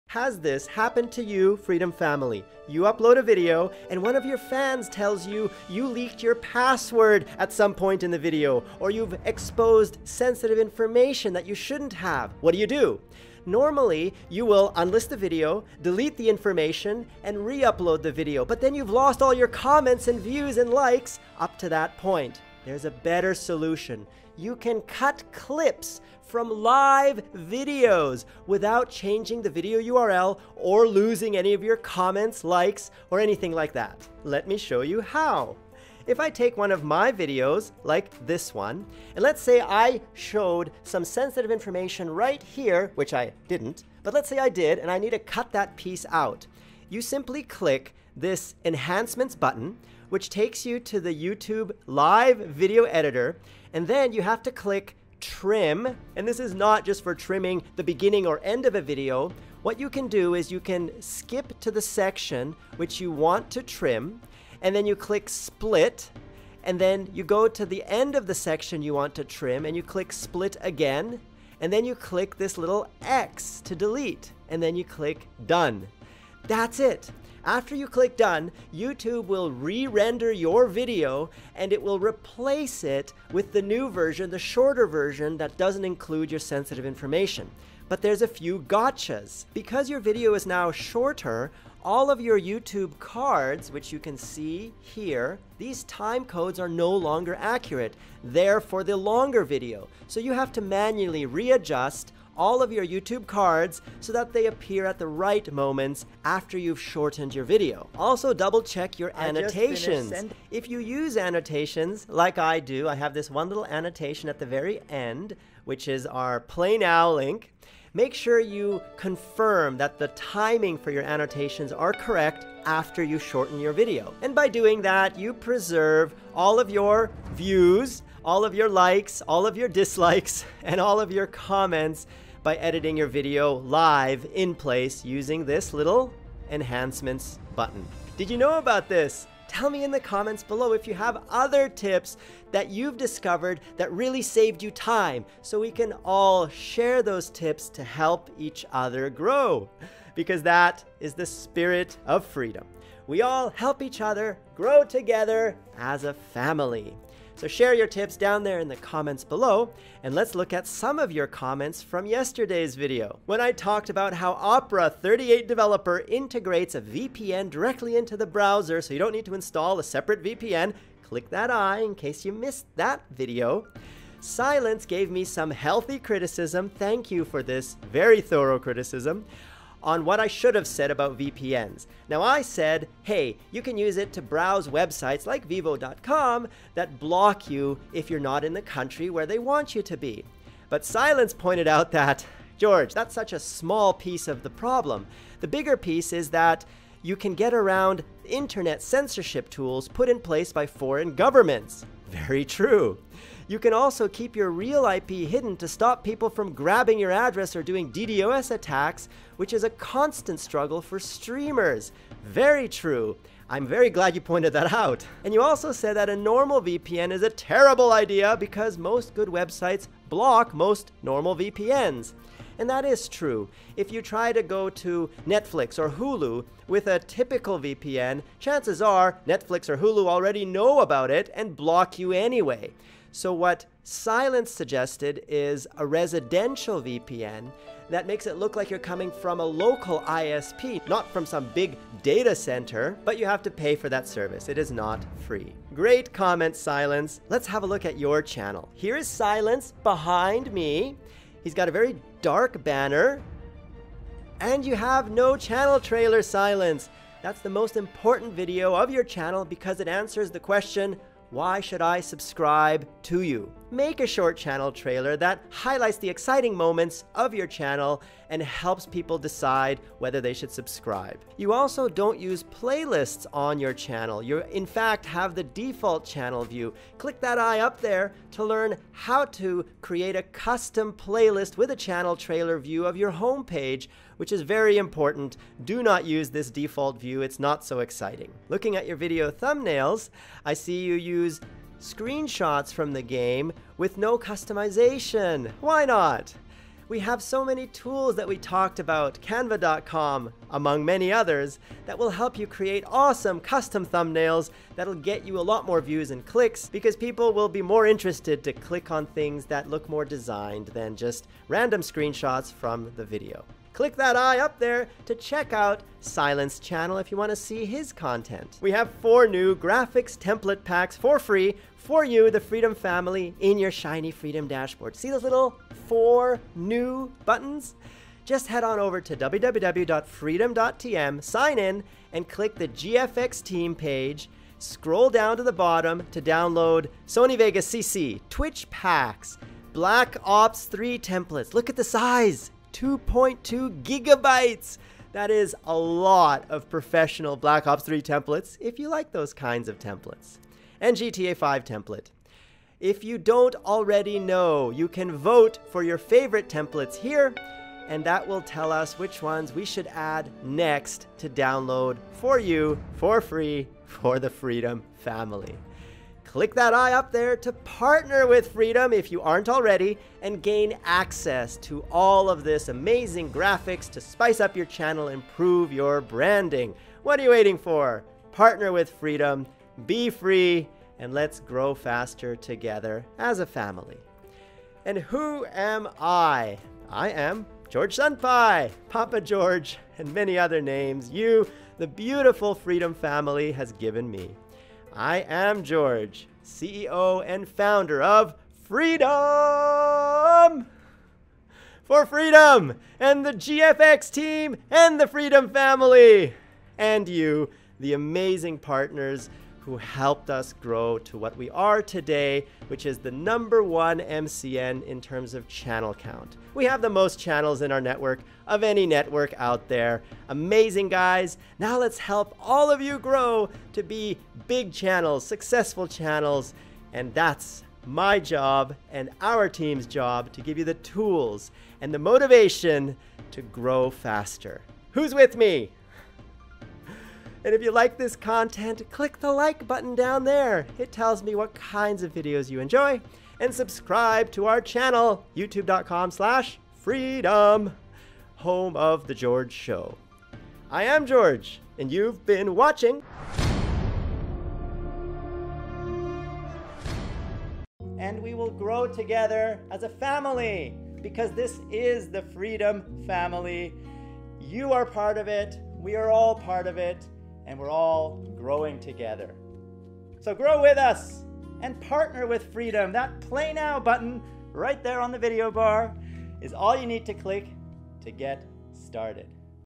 ★ Cut clips from live videos!
You can cut clips from your videos without re-uploading, using the YouTube live video editor!

▼ Links

▼ Heartbeat

▼ Freedom!

▼ Music